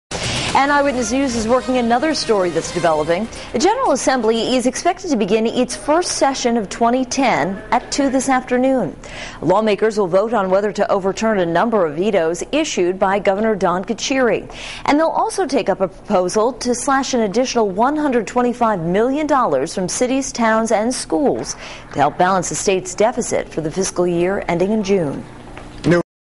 General Assembly kicks off the new year
The General Assembly is expected to begin its first session of 2010 at two this afternoon. Lawmakers will vote on whether to overturn a number of vetoes issued by Governor Don Carcieri. They'll also take up a proposal to slash an additional $125 million dollars from cities, towns and schools to help balance the state's deficit for the fiscal year.